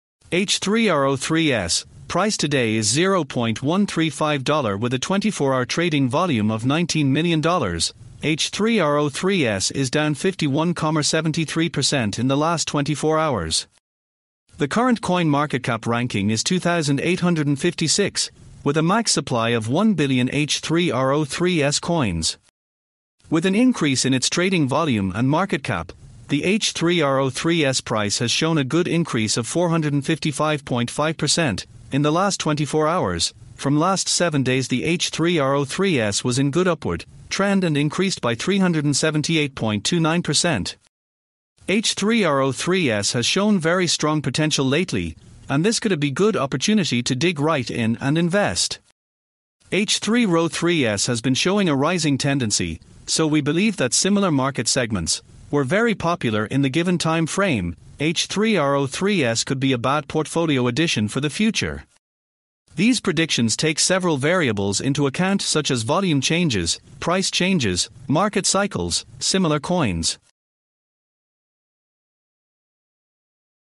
H3RO3S COIN - H3RO3S CRYPTO PRICE PREDICTION 2022 - H3RO3S HUGE PRICE RISE UP +500%
Synopsis: H3 RO 3S, price today is 0.135 $ with a 24-hour trading volume of 19 million $, H3 RO 3S is down 51,73% in the last 24 hours. The current CoinMarketCap ranking is 2856, with a max supply of1 billion H3 RO 3S coins.

With an increase in its trading volume and market cap, the H3 RO 3S price has shown a good increase of 455.5%, in the last 24 hours, From last 7 days the H3 RO 3S was in good upward, trend and increased by 378.29%. H3 RO 3S has shown very strong potential lately, and this could a be good opportunity to dig right in and invest.

H3ro3s has been showing a rising tendency, so we believe that similar market segments, were very popular in the given time frame, H3 RO 3S could be a bad portfolio addition for the future. These predictions take several variables into account such as volume changes, price changes, market cycles, similar coins.